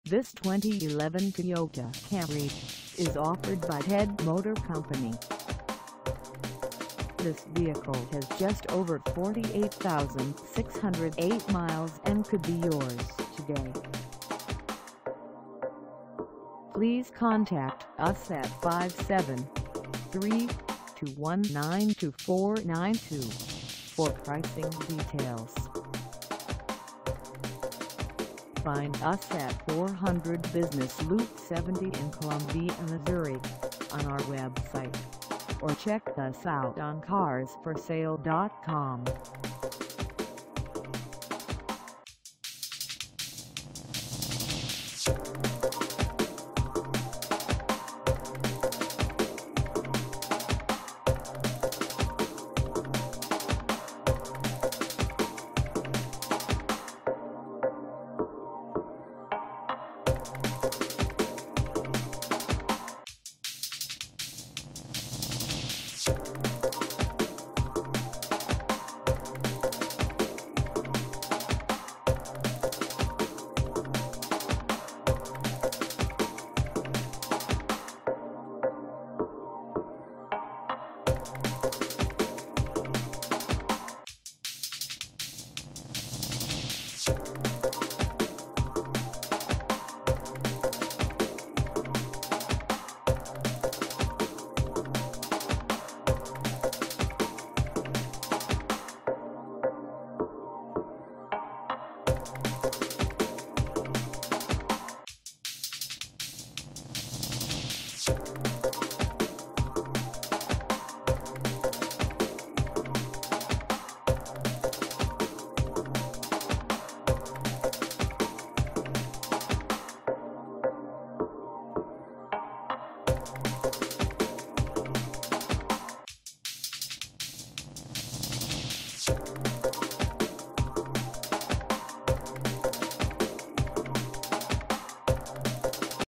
2011 Toyota Camry for sale in Columbia, MO 65203 at the Head
This 2011 Toyota Camry is for sale in Columbia, MO 65203 at Head Motor Company.
Engine: I4 2.5L I4
Transmission:
Color: Red
Mileage: 48608
Contact Head Motor Company 400 Business loop 70  Columbia, MO 65203 to test drive this 2011 Toyota Camry  today.